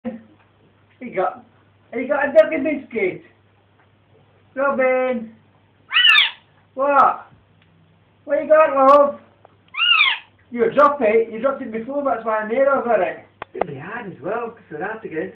Robin eating a dog biscuit
Greedy parakeet steals the biscuit!